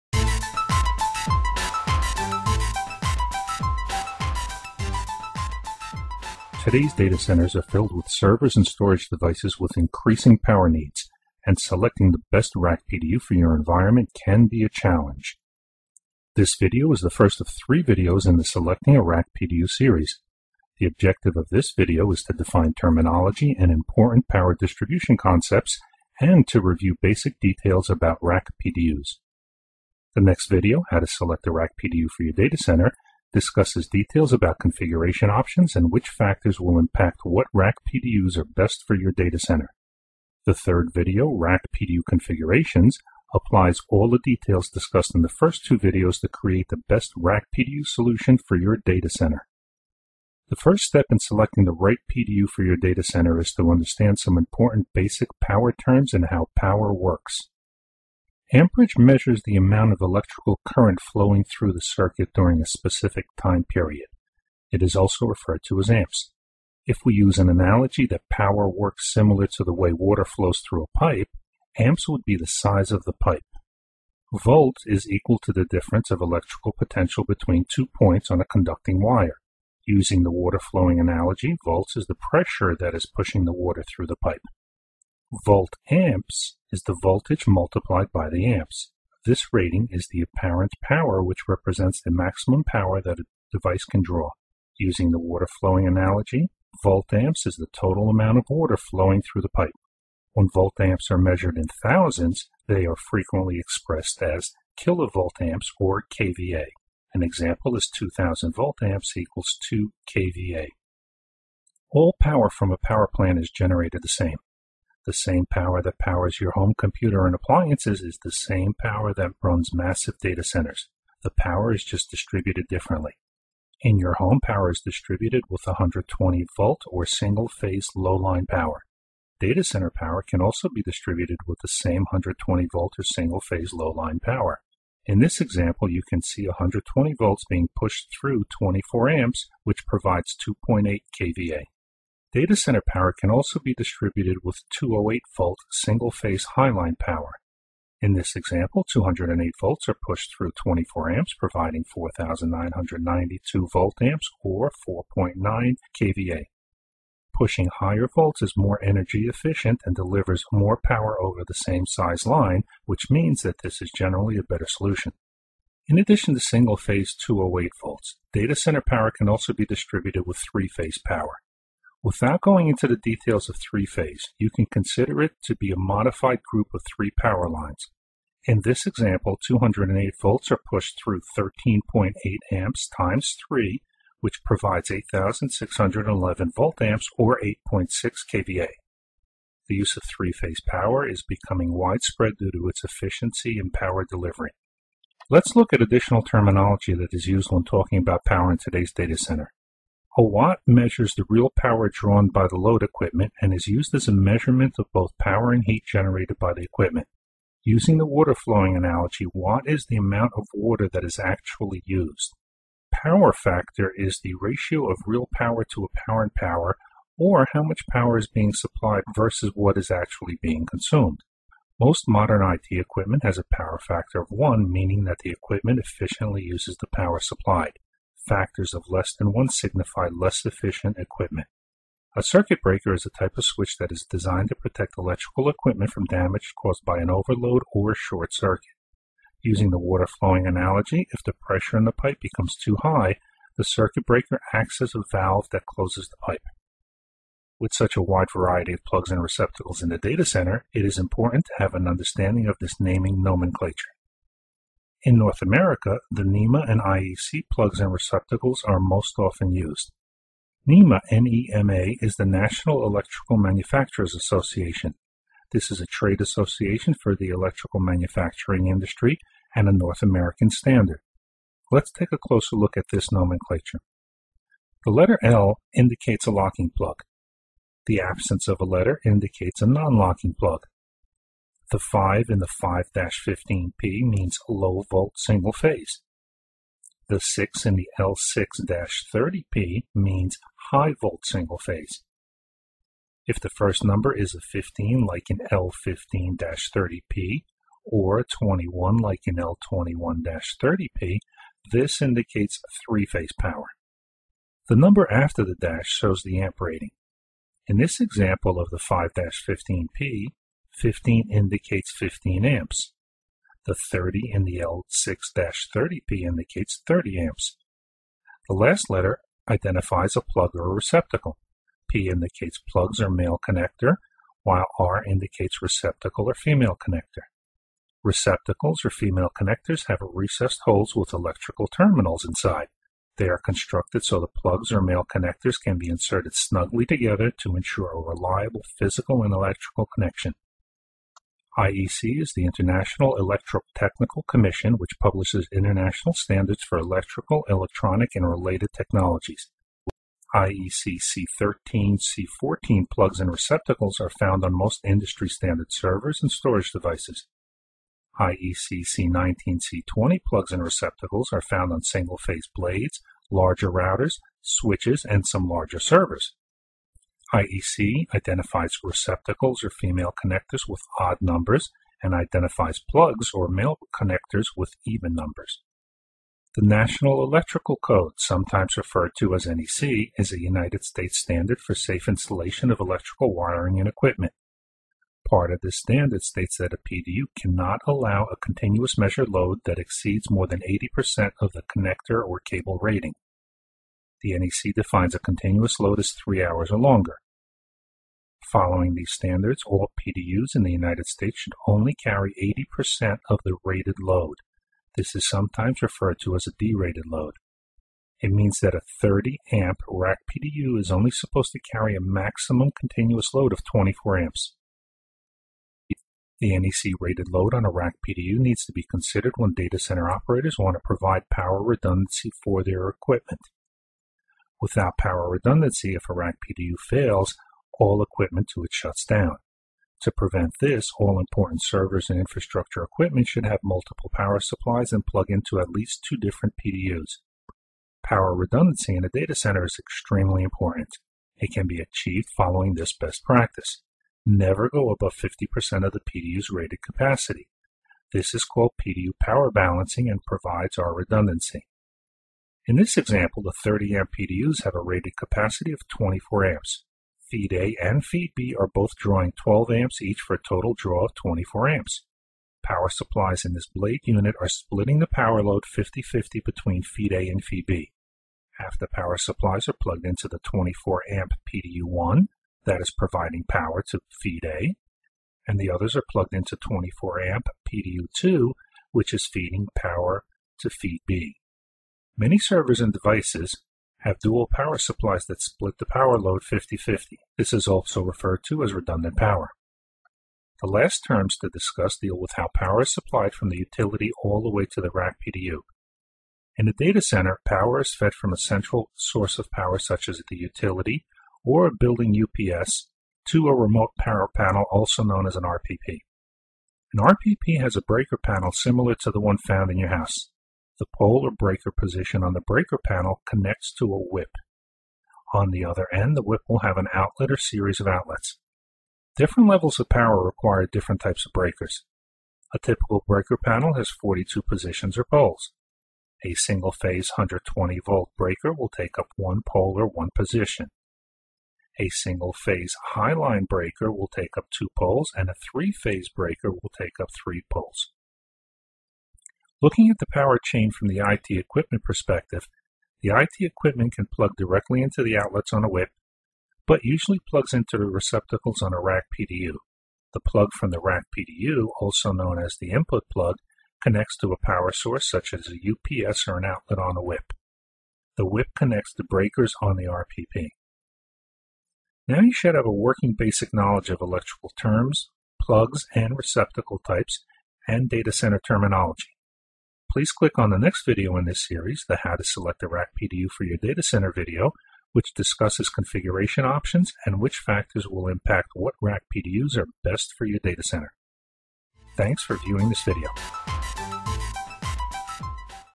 Rack Power (PDU) terms and technology
This video is a primer on the terms and technology behind the rack Power Distribution Units (PDU) that are used in data centers. It's the first of 3 videos that describes how to select a PDU for your data center.